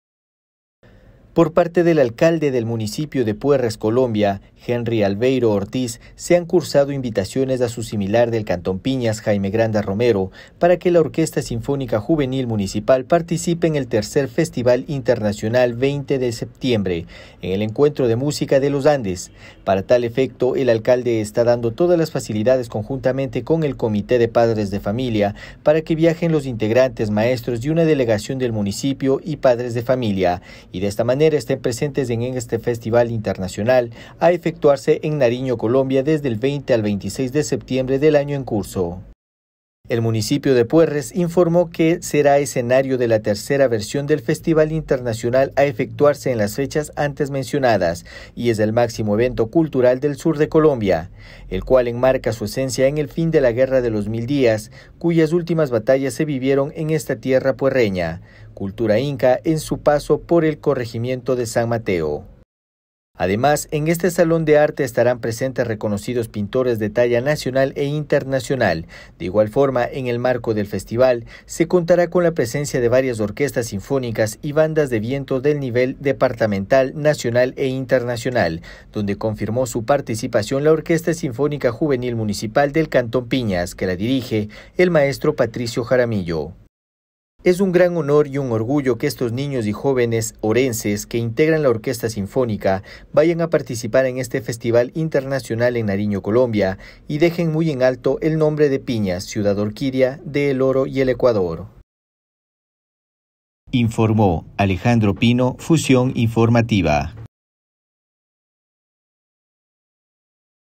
ORQUESTA SINFÓNICA DEL CANTÓN PIÑAS PARTICIPARÁ EN EL FESTIVAL INTERNACIONAL DE ORQUESTAS JUVENILES
LA ORQUESTA SINFÓNICA DEL CANTÓN PIÑAS ESTARÁ PARTICIPANDO EN EL HERMANO PAÍS COLOMBIA EN EL FESTIVAL INTERNACIONAL DE ORQUESTAS JUVENILES, MISMA QUE SE EFECTUARÁ EN NARIÑO DESDE EL 20 AL 26 DEL MES EN CURSO.